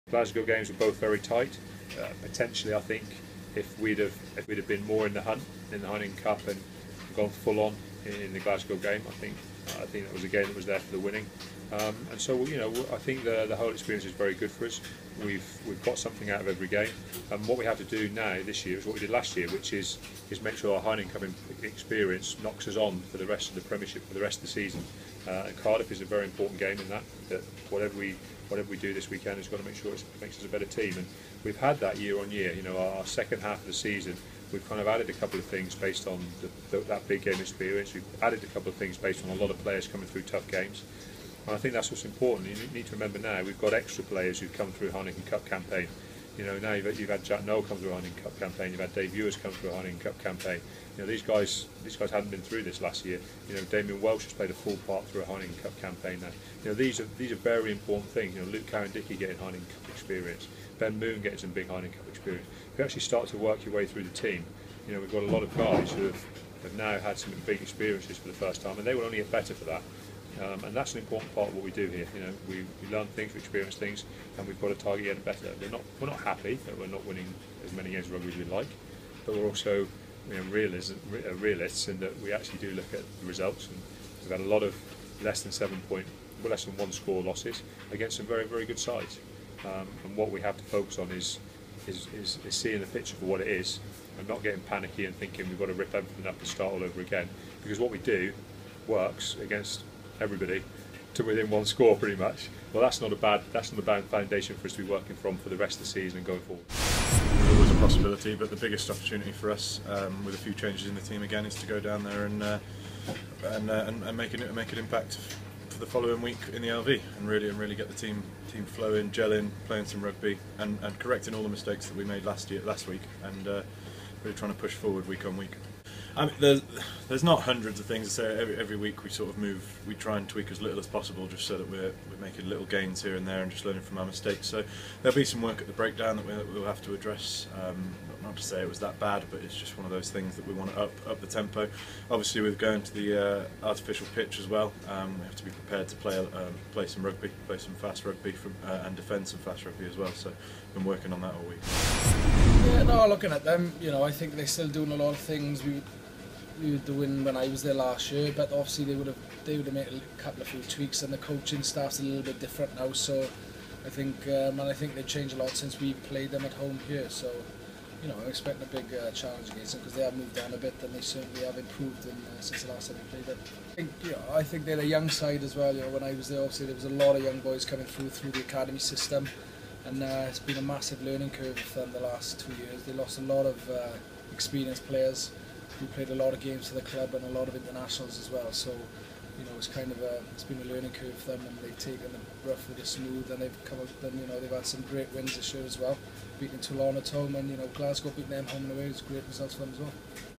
Exeter Chiefs' Rob Baxter, Carl Rimmer and Ceri Sweeney
Exeter Chiefs prop Carl Rimmer is targeting a win in the Heineken Cup to launch his side towards a successful second half of the season.

The Devon club have struggled for form in recent months, winning just one of their past eight games in all competitions.

However, Rimmer feels a victory in their final Pool Two match against Cardiff Blues this weekend will help them reverse the trend ahead of crucial games in the LV= Cup and the Premiership.